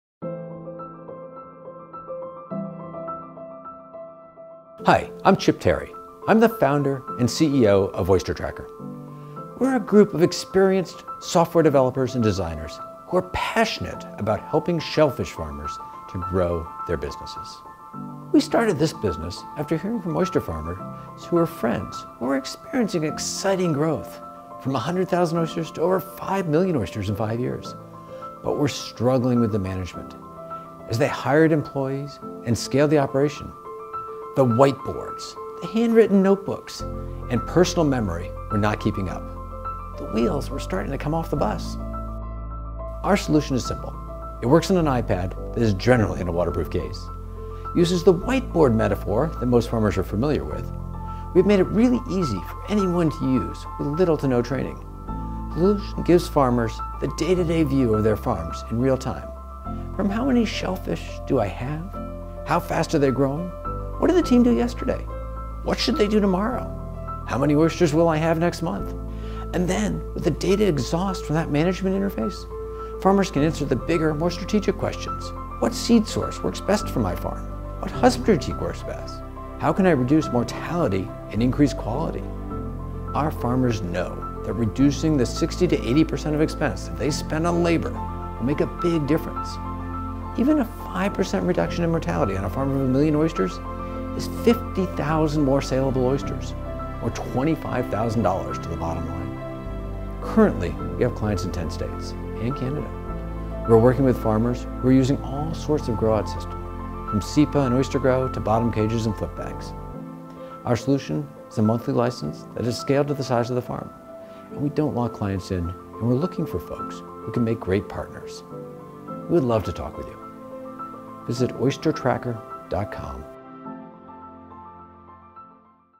OYSTER TRACKER
Oyster Tracker is a farm management solutions for shellfish farms. We're optimizing husbandry for the growing, global aquaculture industry. We are building the simplest aquaculture management platform; It’s like a second brain that helps oyster farmers big and small manage their workers, optimize their lease activities, and plan their harvests.